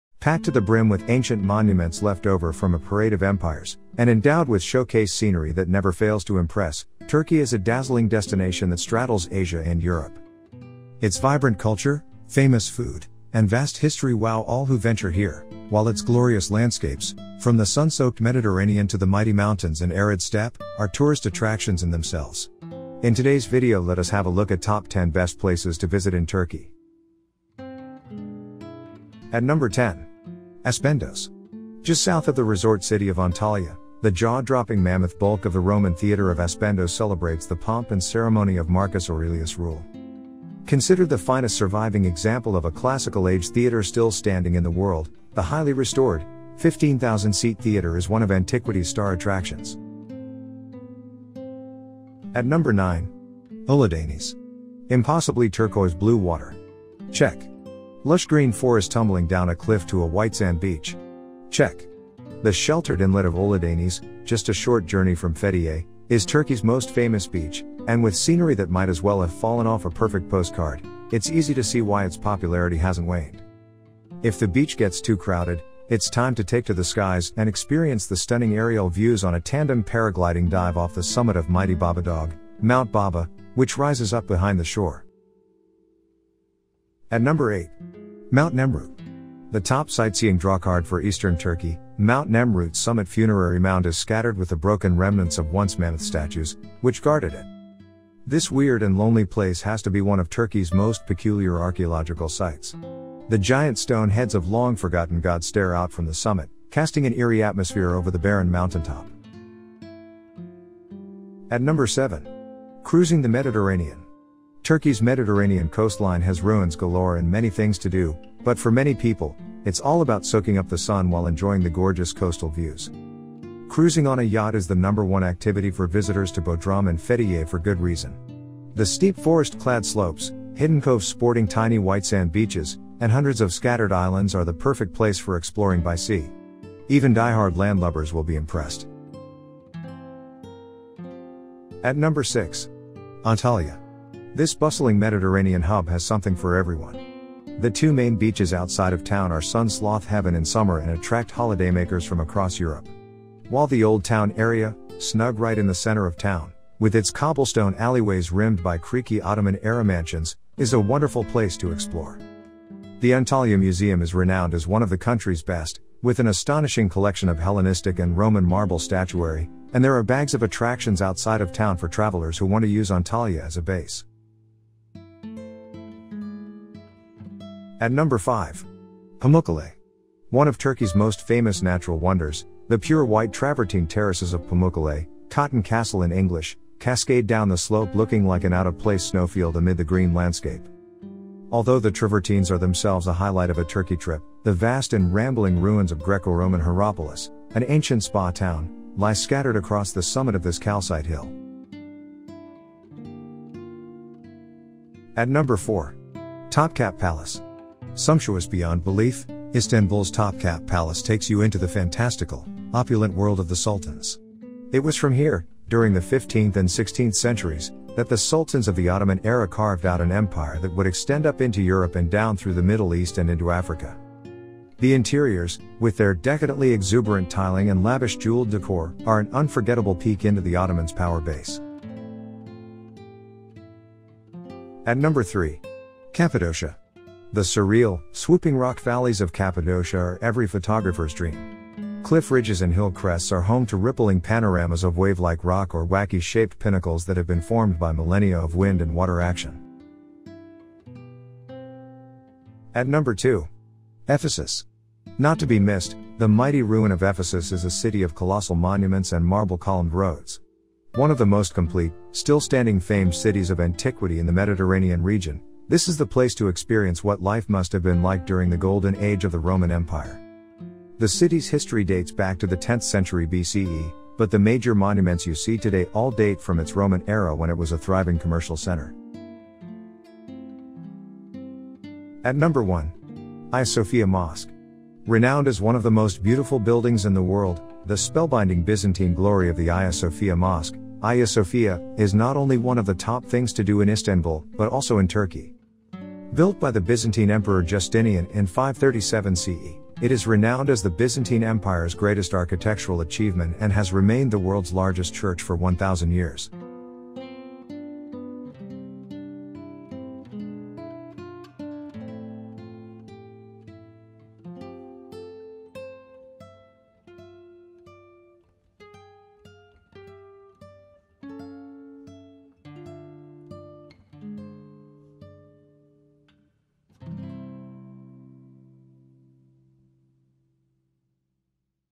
Top 10 Best Places to visit in Turkey I Turkey Travel Destinations
Hello everyone! Welcome back to the Dope Tourist YouTube channel. In Today's Video let us have a look at the Top 10 Best Places to visit in Turkey I Turkey Travel Destinations.

Turkey and attractions It is my pleasure to present to all the tourists "Top 10 Places to Visit in Turkey - Travel Destinations in Turkey".

Packed to the brim with ancient monuments left over from a parade of empires, and endowed with showcase scenery that never fails to impress, Turkey is a dazzling destination that straddles Asia and Europe.

Its vibrant culture, famous food, and vast history wow all who venture here, while its glorious landscapes, from the sun-soaked Mediterranean to the mighty mountains and arid steppe, are tourist attractions in themselves. In today's video let us have a look at Top 10 Best Places to visit in Turkey.

Hope you all will enjoy the video very much. Thank you so much for being with us.

0:00 Introduction
0:32 ASPENDOS AT NUMBER 10
0:56 OLUDENIZ AT NUMBER 9
1:30 MOUNT NEMRUT AT NUMBER 8
1:54 CRUISING THE MEDITERRANEAN AT NUMBER 7
2:23 ANTALYA AT NUMBER 6
3:04 PAMUKKALE AT NUMBER 5
3:30 TOPKAPI PALACE AT NUMBER 4
3:58 CAPPADOCIA AT NUMBER 3
4:15 EPHESUS AT NUMBER 2
4:47 HAGIA SOPHIA MOSQUE AT NUMBER 1

➤Suggested Short Videos:-